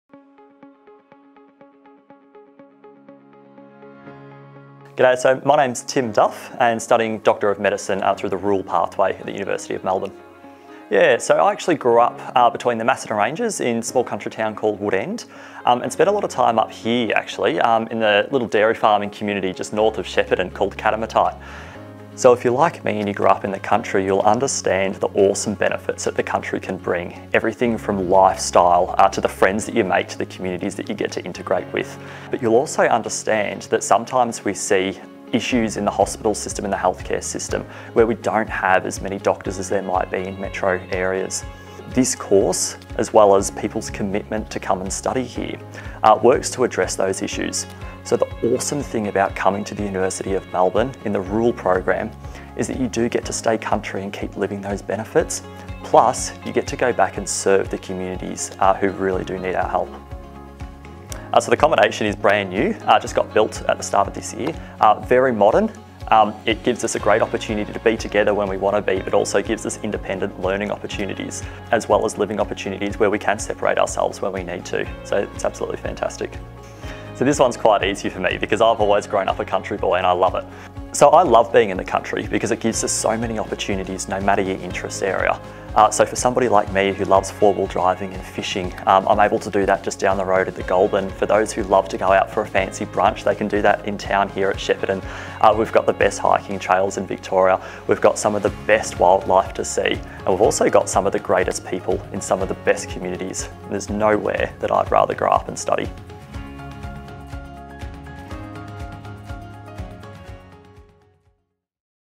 Meet Tim Duff, Doctor of Medicine Rural Pathway Student
For Tim, there’s nowhere he would rather grow up and study than in the country. Having the ability to go four-wheel driving, hiking, or dining in Shepparton’s town centre while serving the community as a doctor is ideal for Tim.

The new accommodation facilities on-campus mean that students can come together and learn from each other, but also have their own independent spaces to live.